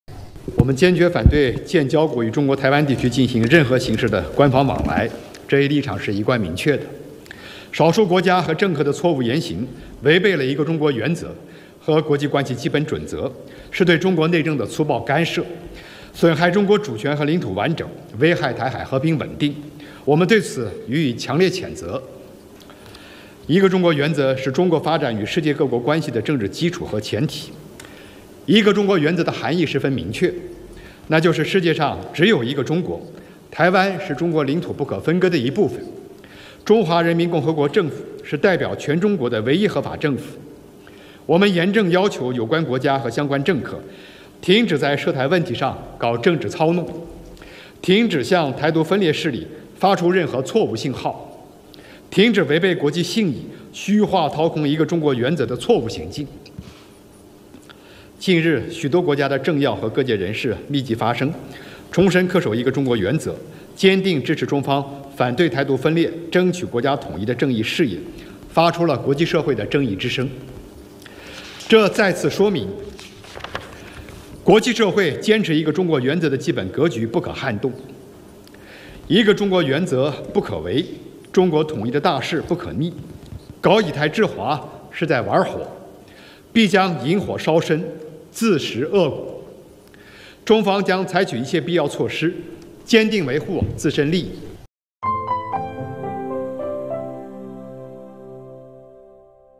中方：坚决反对建交国与中国台湾地区进行任何形式的官方往来
有记者提问：我们注意到，有少数中国建交国的议员、前政要等参加了赖清德的“就职仪式”或对赖表达了“祝贺”，中方对此有何评论？

汪文斌：我们坚决反对建交国与中国台湾地区进行任何形式的官方往来，这一立场是一贯的、明确的。少数国家和政客的错误言行违背了一个中国原则和国际关系基本准则，是对中国内政的粗暴干涉，损害中国主权和领土完整，危害台海和平稳定，我们对此予以强烈谴责。

一个中国原则是中国发展与世界各国关系的政治基础和前提。一个中国原则的含义十分明确，那就是世界上只有一个中国，台湾是中国领土不可分割的一部分，中华人民共和国政府是代表全中国的唯一合法政府。

我们严正要求有关国家和相关政客，停止在涉台问题上搞政治操弄，停止向“台独”分裂势力发出任何错误信号，停止违背国际信义、虚化掏空一个中国原则的错误行径。

近日，许多国家政要和各界人士密集发声，重申恪守一个中国原则，坚定支持中方反对“台独”分裂、争取国家统一的正义事业，发出了国际社会的正义之声。这再次说明，国际社会坚持一个中国原则的基本格局不可撼动。一个中国原则不可违，中国统一的大势不可逆，搞“以台制华”是在玩火，必将引火烧身、自食苦果。中方将采取一切必要措施坚定维护自身利益。